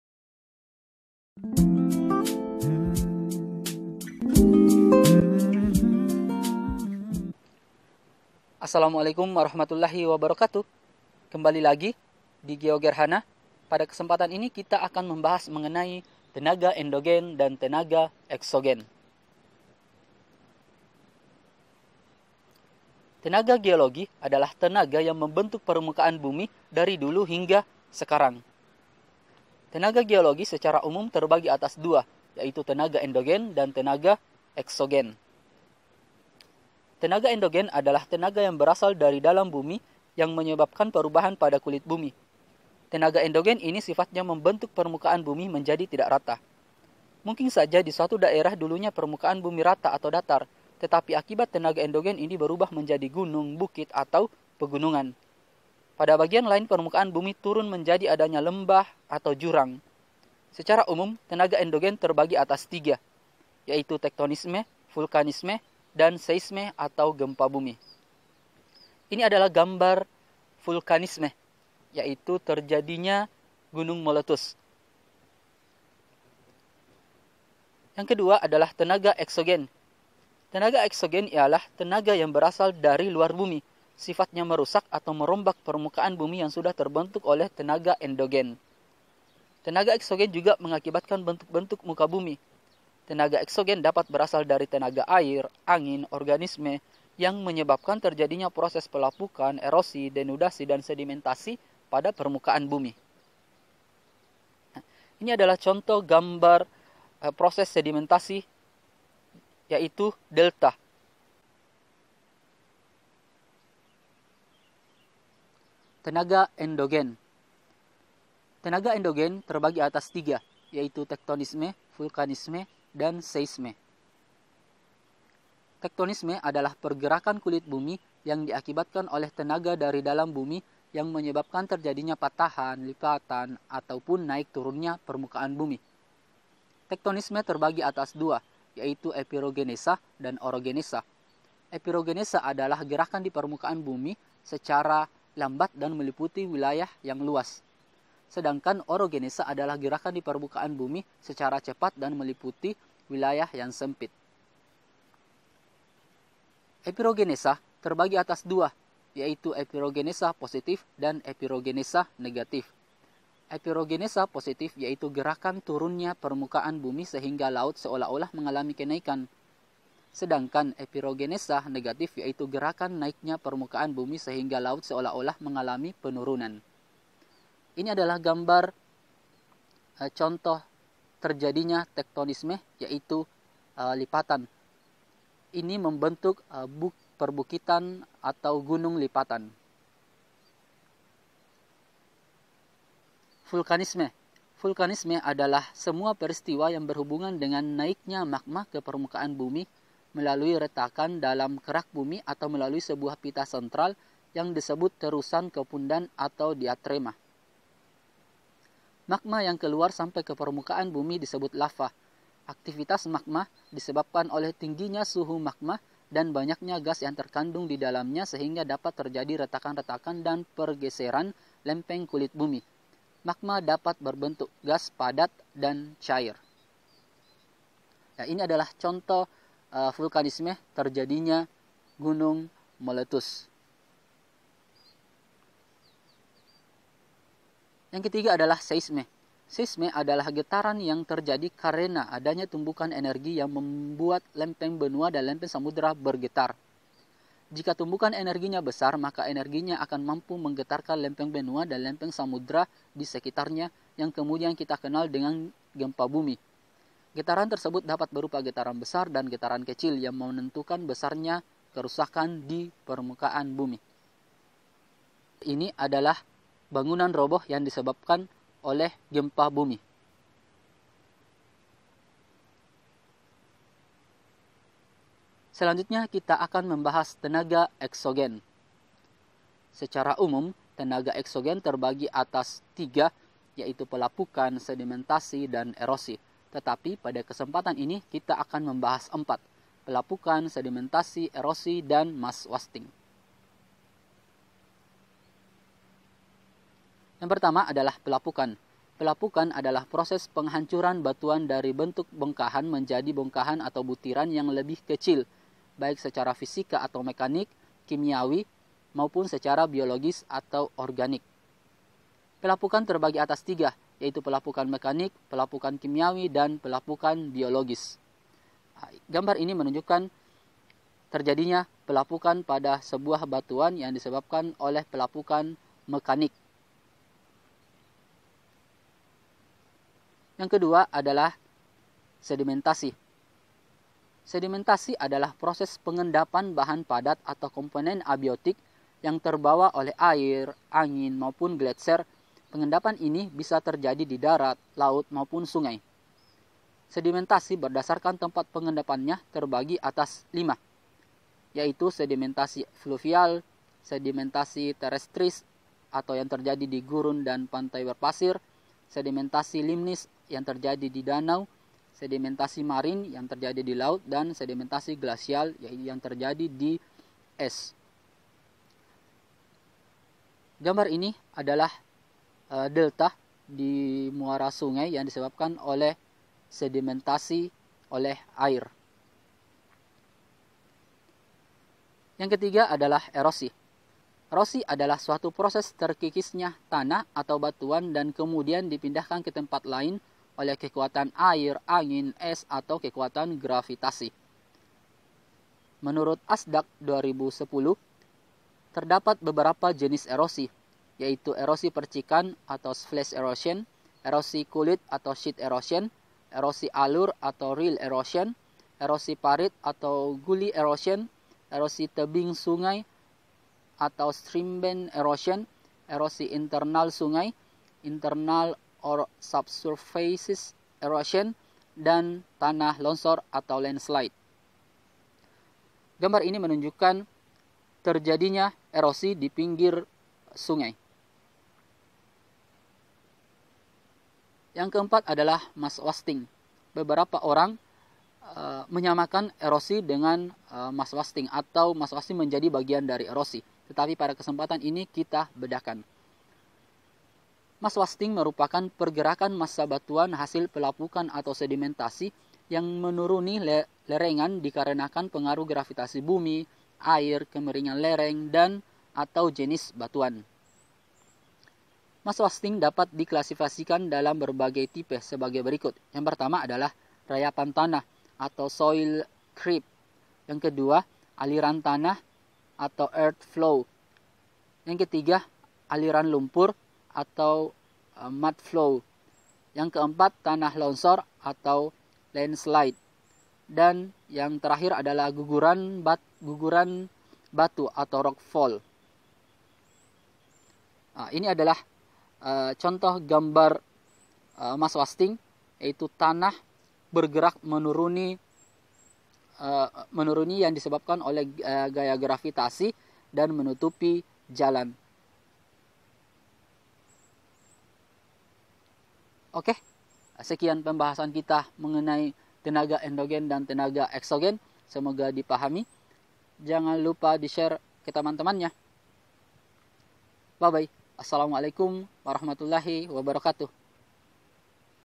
Tenaga Endogen dan Eksogen
Tenaga endogen
1. Tektonisme
2. Vulkanisme
3. Seisme

Tenaga eksogen
1. Pelapukan
2. Sedimentasi
3. Erosi
4. Masswasting